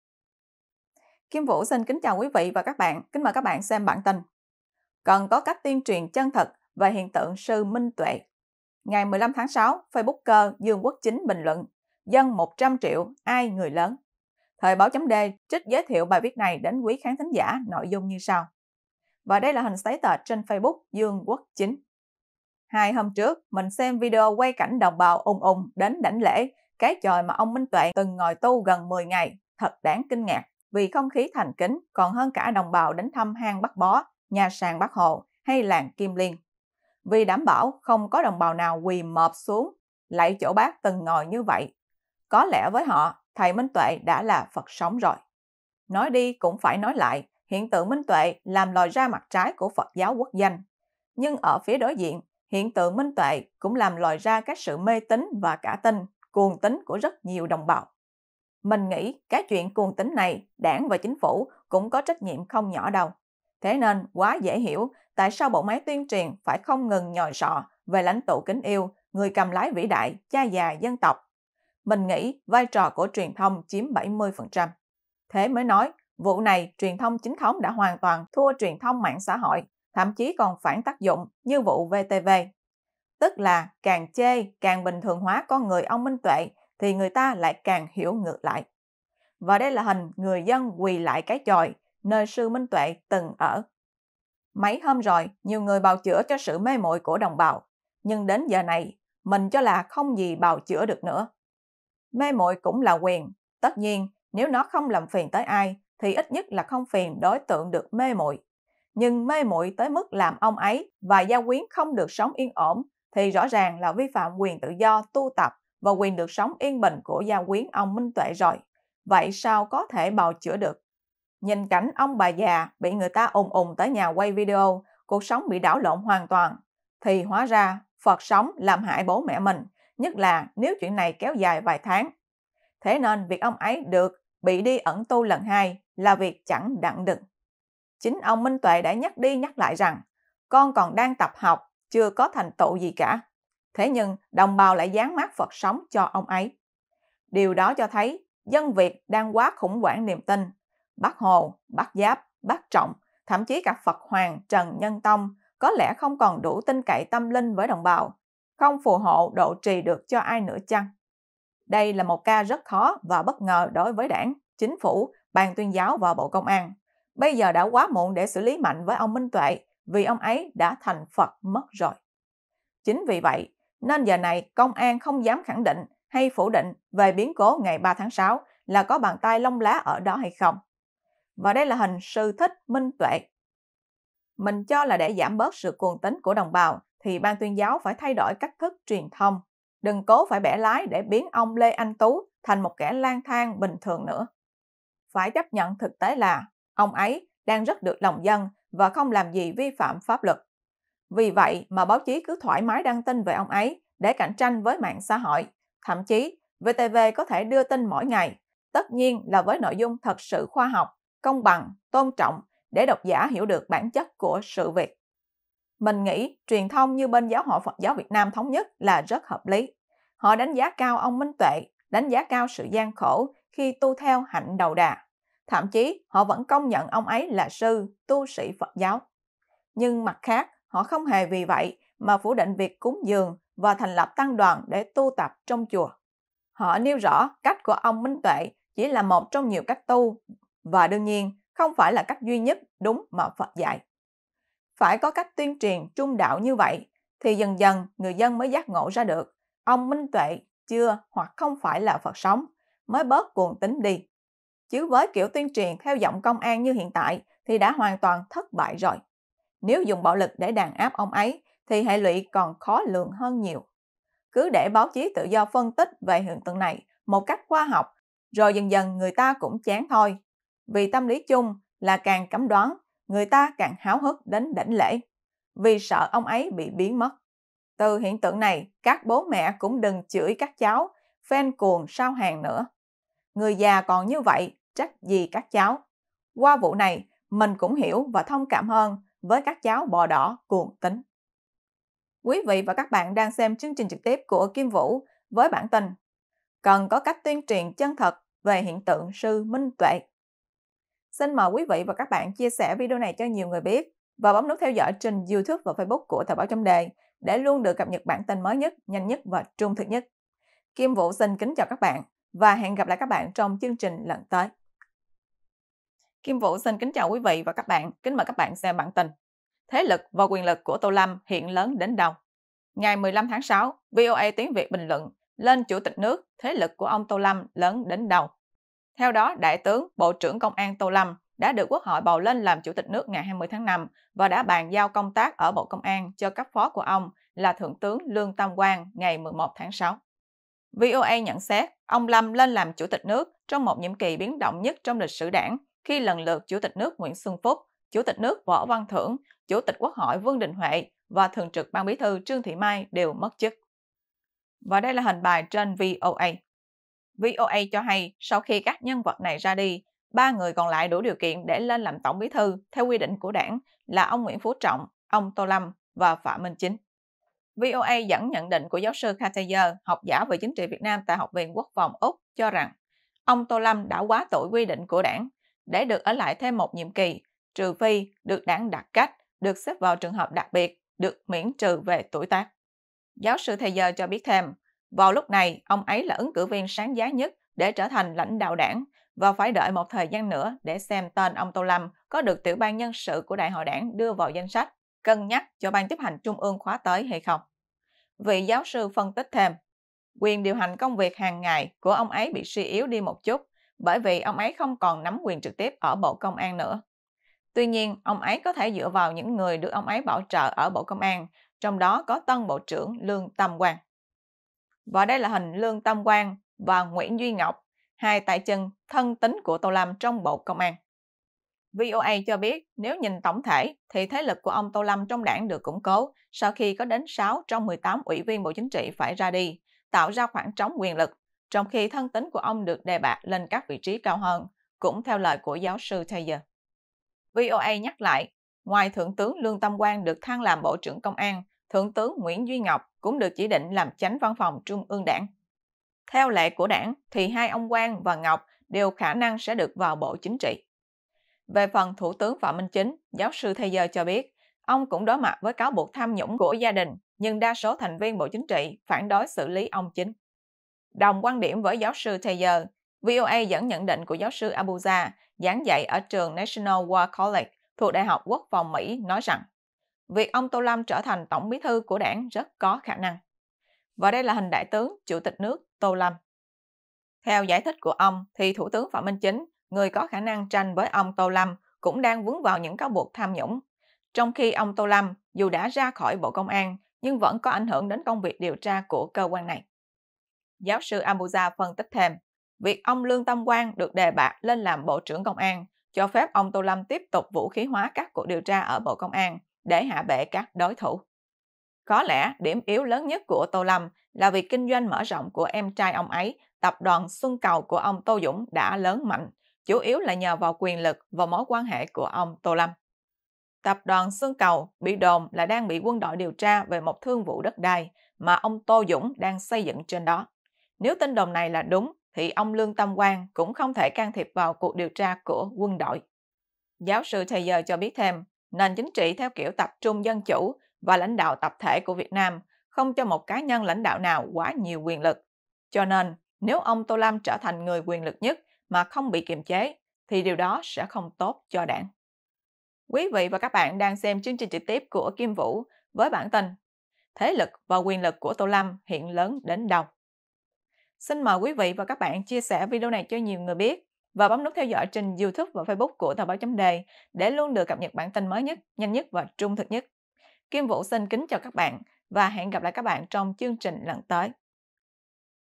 Dân trăm triệu ai người lớn?
Hiện tượng sư Minh Tuệ làm lòi ra mặt trái của Phật giáo quốc doanh. Nhưng ở phía đối diện, hiện tượng này cũng làm lòi ra cái sự mê tín và cả tin, cuồng tín của rất nhiều đồng bào. Vì vậy, Ban Tuyên giáo cần thay đổi cách thức truyền thông. Đừng cố phải bẻ lái để biến ông Lê Anh Tú thành 1 kẻ lang thang bình thường nữa. Phải chấp nhận thực tế là, ông ấy đang rất được lòng dân và không làm gì vi phạm pháp luật, nhưng cách tu của ông chỉ là một trong nhiều cách tu mà Phật dạy, và ông không phải là Phật sống.